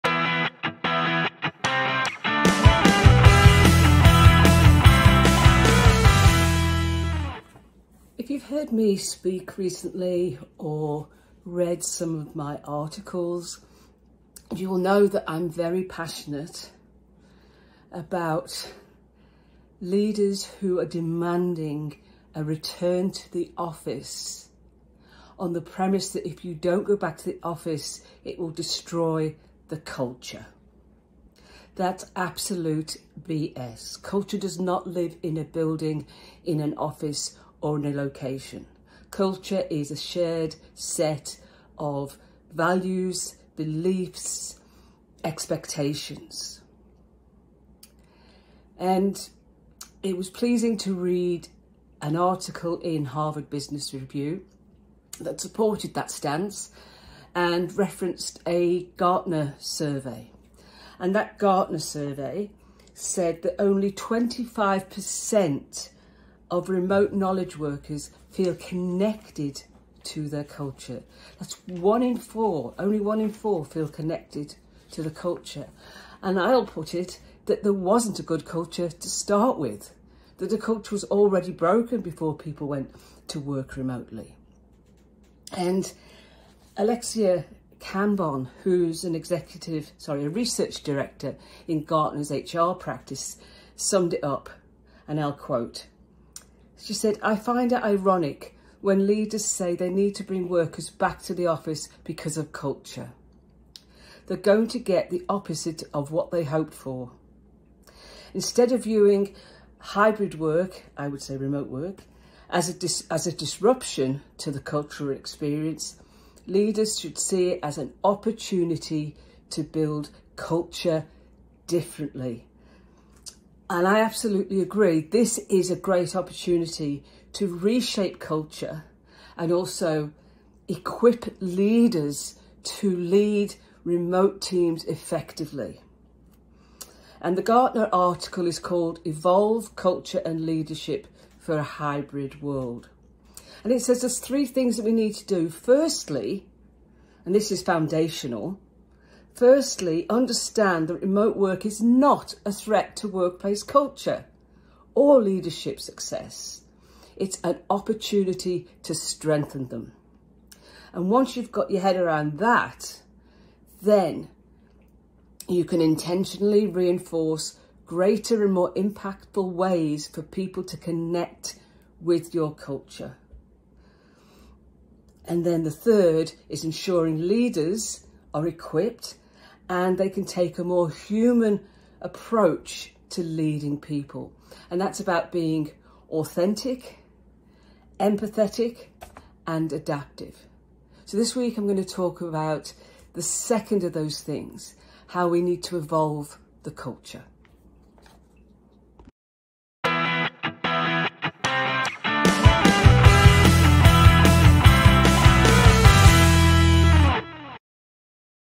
Reshaping Culture - 1. Remote Work Not A Threat
Leaders must understand that remote work does not threaten workplace culture or leadership success.

Make the culture thrive across a distributed workforce through alignment and connectedness